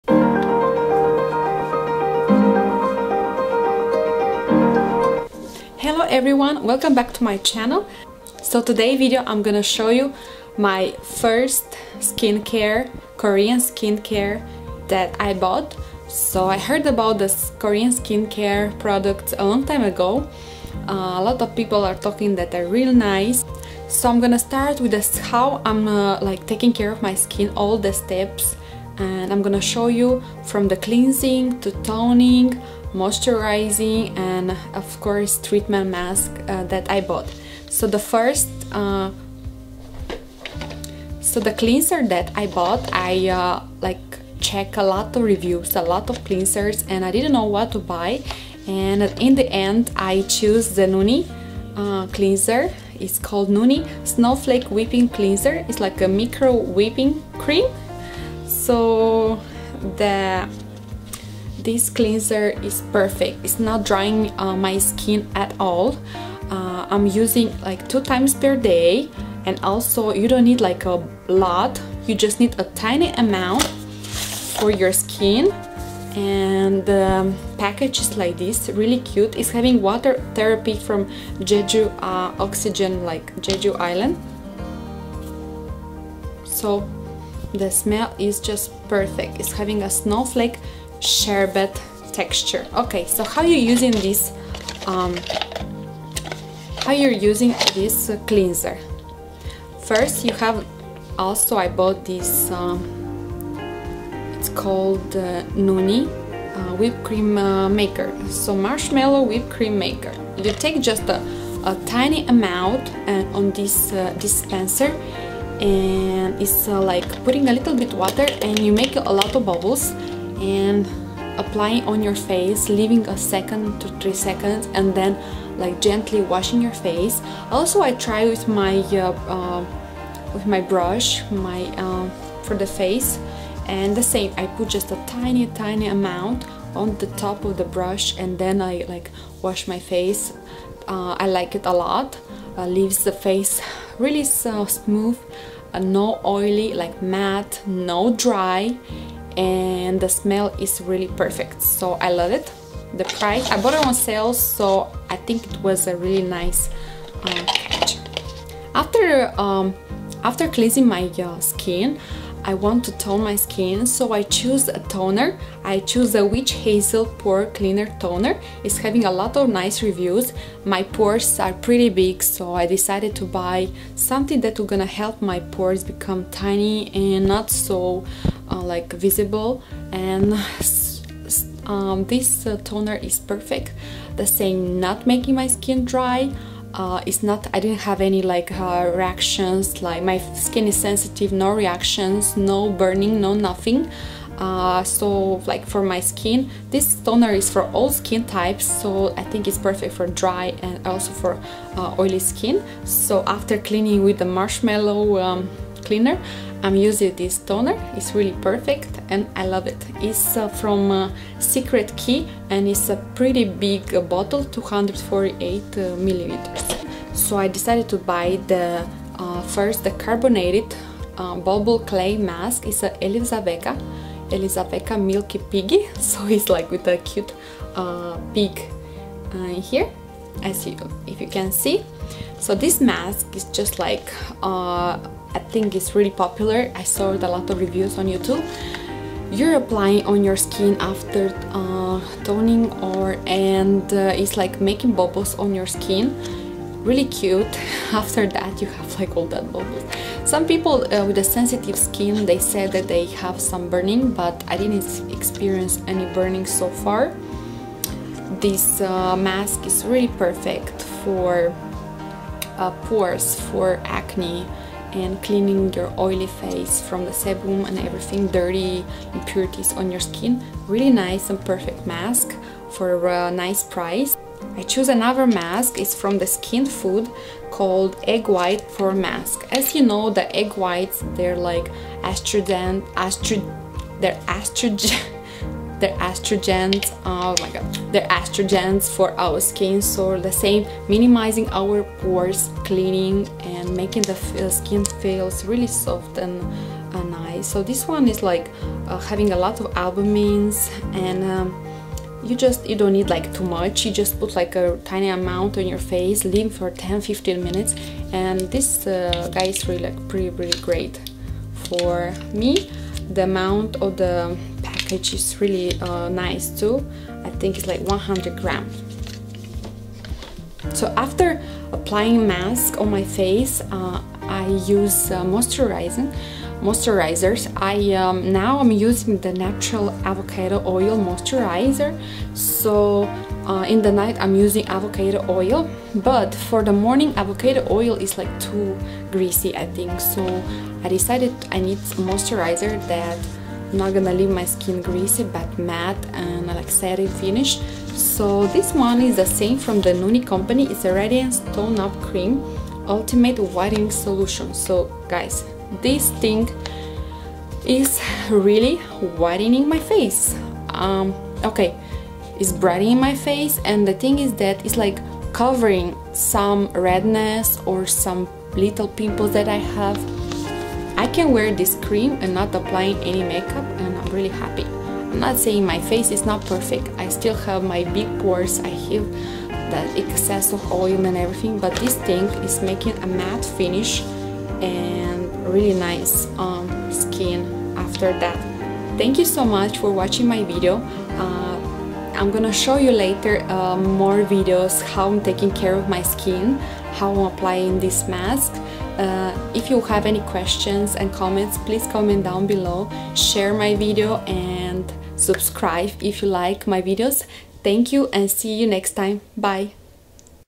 Korean Skincare Haul❤ Spring everyday routine
Hello Beauties!!
I thought to show you my new skincare regimen.The Spring Skincare that I love and use everyday!!
I recently bought some Korean Beauty  Skincare products
From Cleanser to masks
Its for my Oily combination skin,and for my huge pores
I bought them online ,and I choose more products based on good reviews.
I start loving this products so much,They make my skin look better and not so many blemishes.
Also my pores they minimize a little
I am cleaning my face with the Cleansear and a face brush,make a very big difference,Skin so soft ,clean and not drying to much.
Then applying my toner
2 times per week I choose a mask too
And I finish with the eye cream and moisturiser